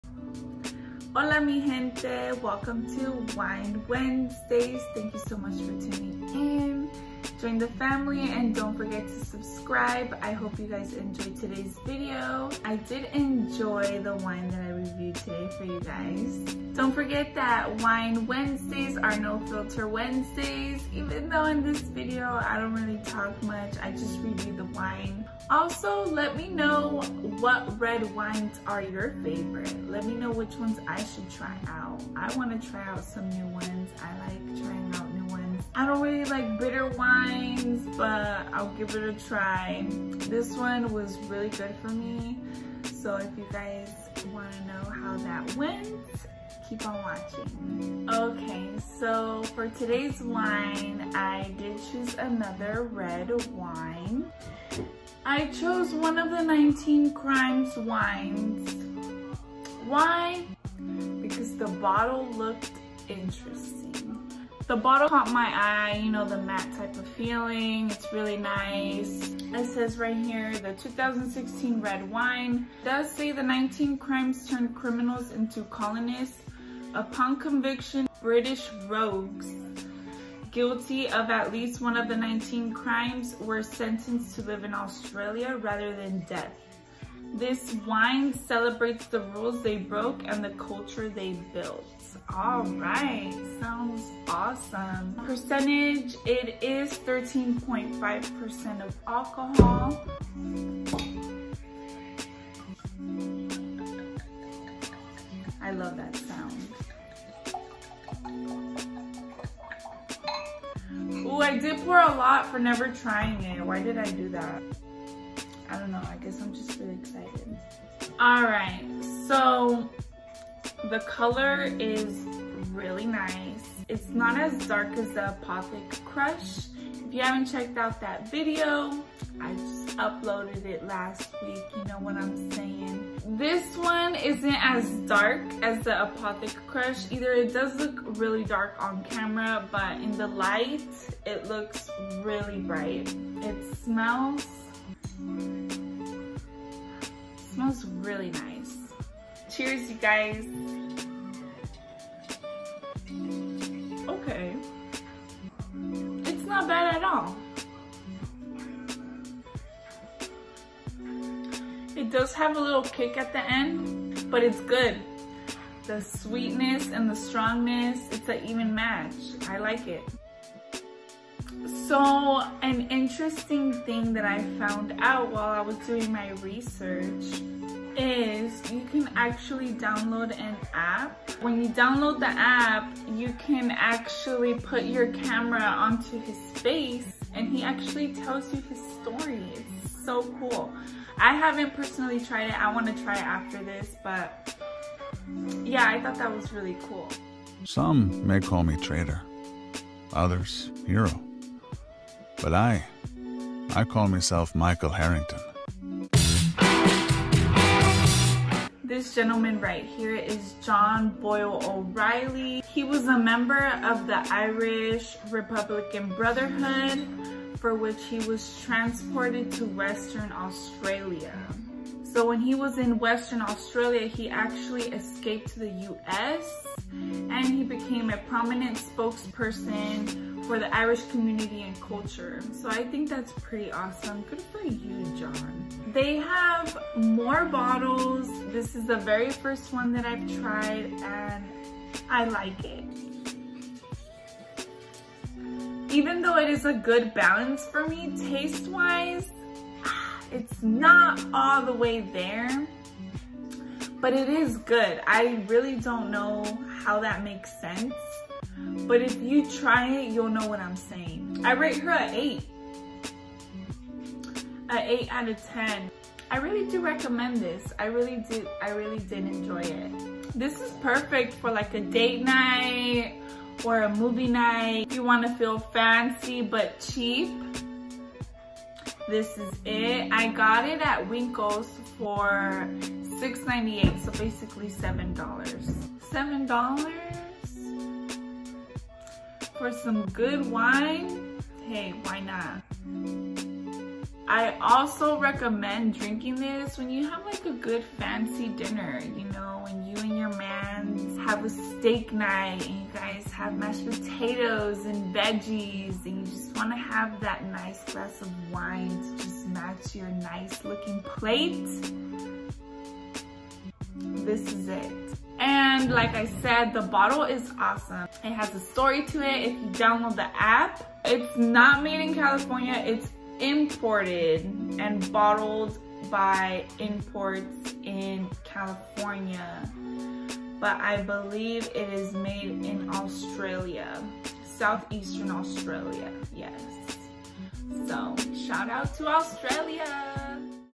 WINE WEDNESDAYS WITH TALLASSJASMINEE_ | 19 CRIMES WINE REVIEW
hello guys and thanks so much for tuning into another Wine Wednesday here with me! I did review the 19 crimes red wine and it was good to me.
**BY ANY MEANS I AM NOT A PROFESSIONAL WINE TASTER. I DO THIS FOR FUN AND TO TASTE DIFFERENT WINES BECAUSE I AM A HUGE WINE FAN**
When I mention towards the end that the taste was not all the way there for me, it has a small taste of oak lingering around in your palate, it didn’t bug me but like I said.. it’s there.

In this weeks videos I did not add the mini bloopers, let me know if you guys want me to keep those or take those off.
-do you like the videos with the bloopers or without??🤷🏽‍♀️

Give me some red wine recommendations! I’ll give it a try and give you a shoutout as well for the awesome recommendation ❤️

I did download a new editing app so just give me some time to play with it, the edits in the future will be better! :)
CAPTURING AND EDITING BY: Myself, all done on my iPhone XR.

SOUND BY: the one and only Augusto Delgado, thanks so much 🙏🏽